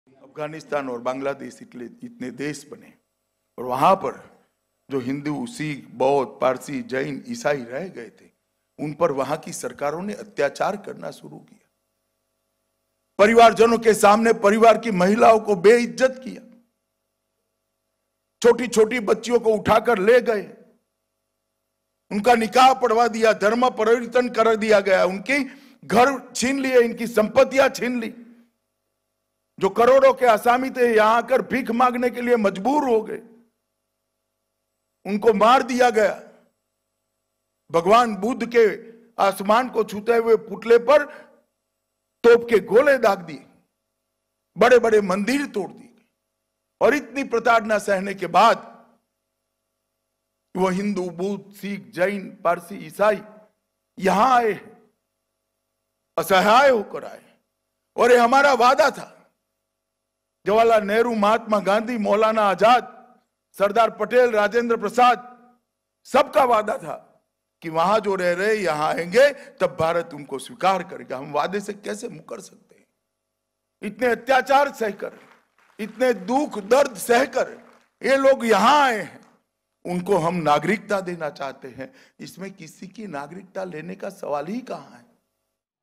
किसी की नागरिकता छीनने का सवाल ही नहीं : AMIT SHAH | R9TV
रायपुर में मध्य क्षेत्रीय परिषद की बैठक में अमित शाह ने कहा की, जो वादा जवाहरलाल नेहरू ,महात्मा गाँधी और मौलाना आज़ाद ने किया था हम वो पूरा कर रहे हैं